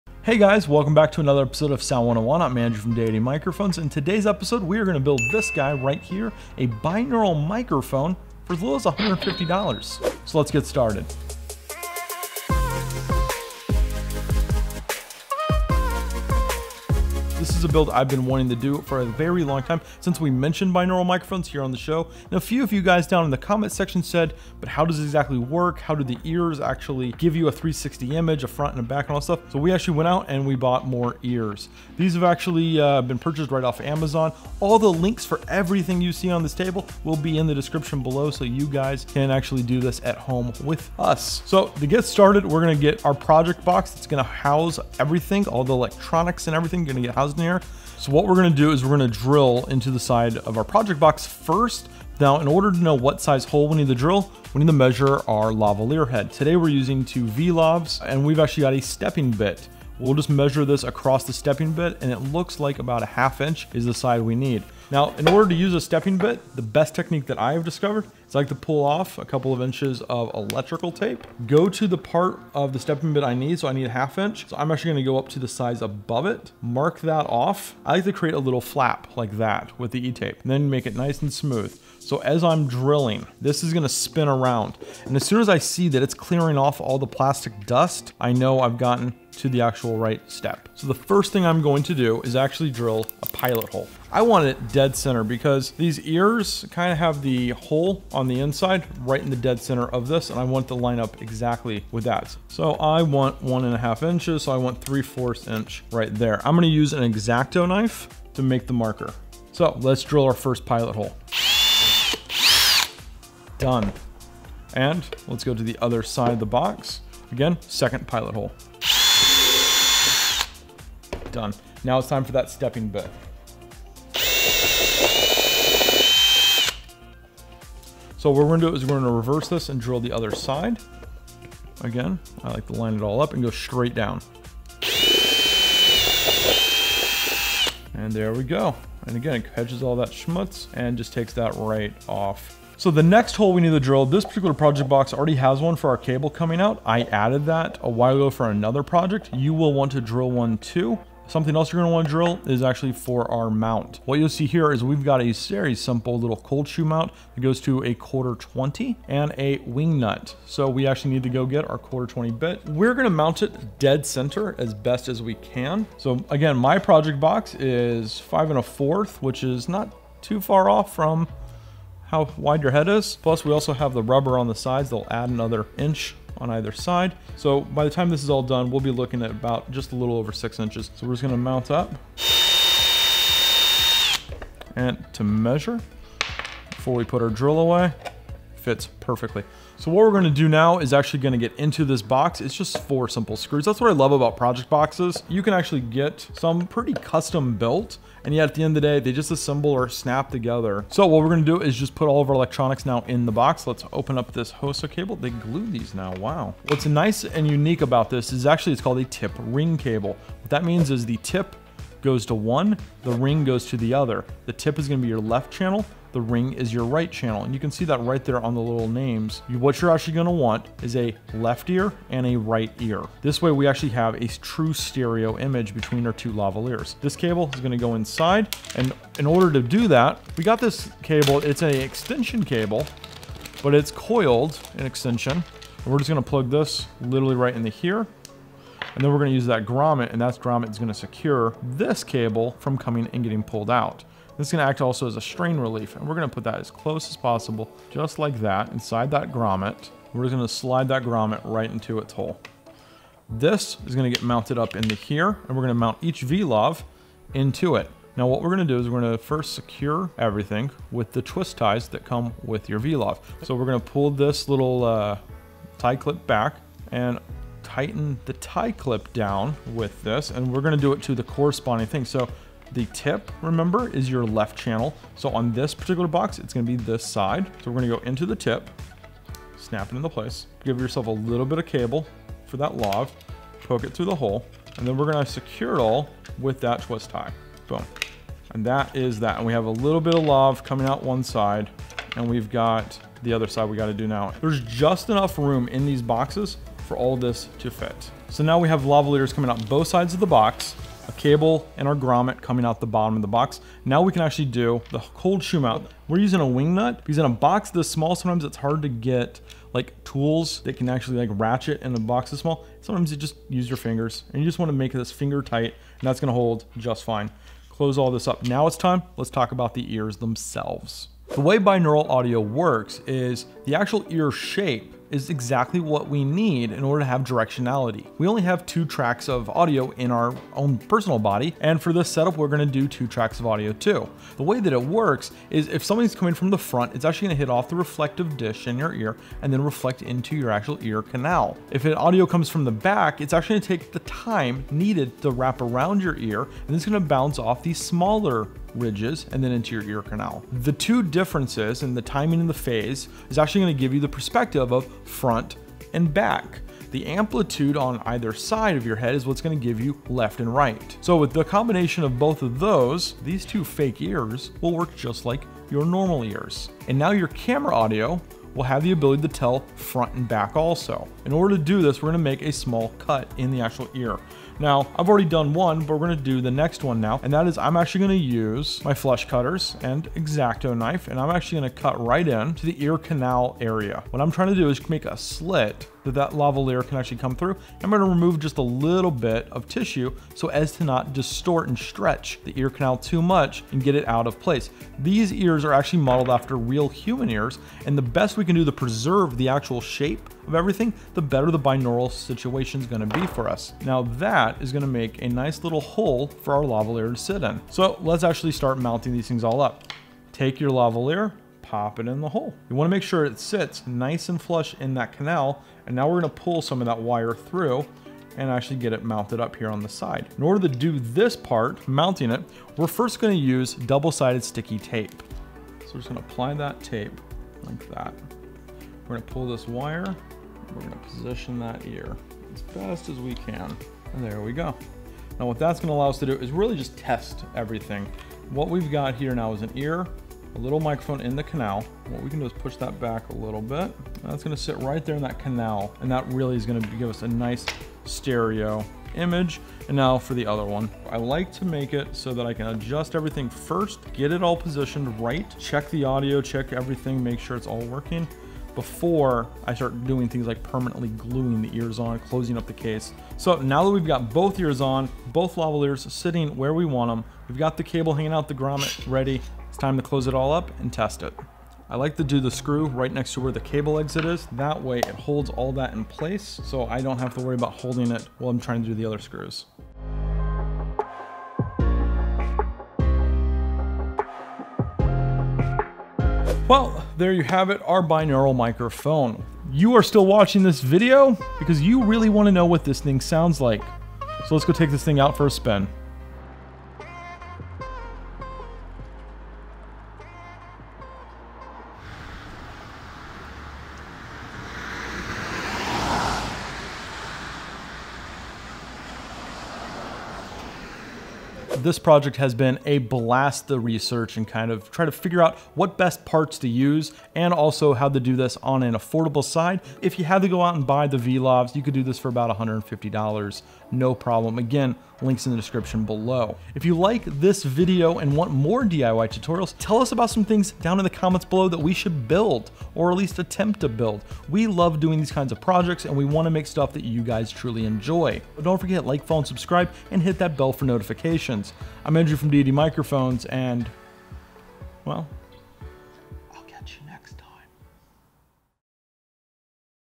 DIY Microphone for ASMR!
Curious about immersive audio for 360° video or ASMR? Don't want to break the bank? Andrew has a budget build for you on this episode of Sound 101! Learn how to build a DIY microphone for ASMR videos for only about $150!

We use two of our very own Deity V.Lavs and a few cheap components to create a fully immersive audio capturing machine that will take your ASMR /360VR to the next level! Get compelling audio for any of your projects using only this build and your DSLR or stereo pocket recorder.

Links:
Pair of Human Ears $22
HOSA Splitter $5
Coiled 3.5mm Extensions $8
2x Deity V.Lav $49
Cold Shoe Mount $5
Project Box $10